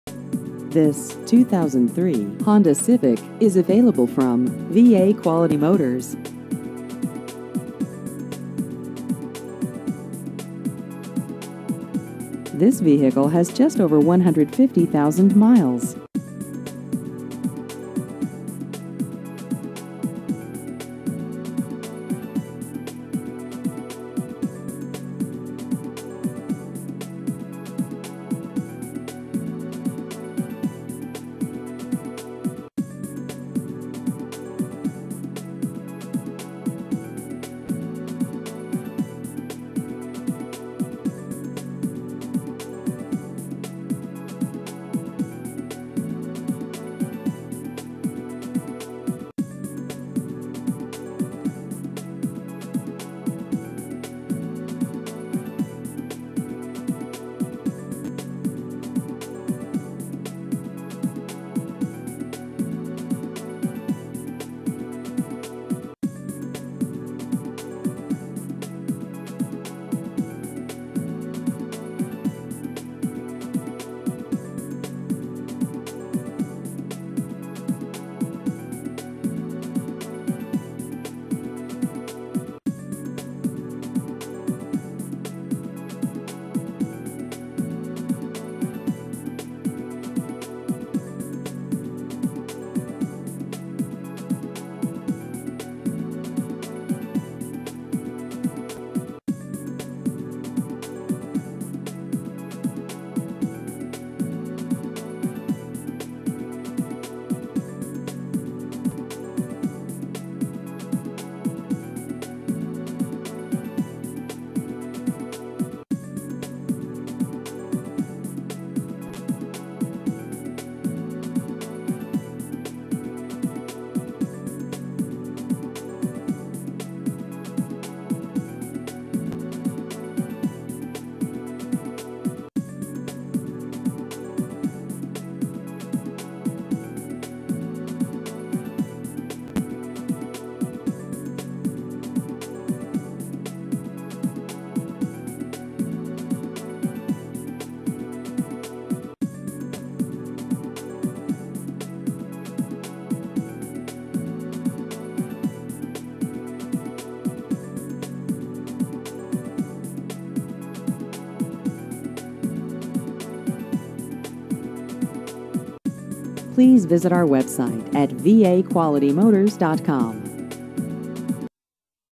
2003 Honda Civic Used Cars Louisville KY
This 2003 Honda Civic is available from VA Quality Motors.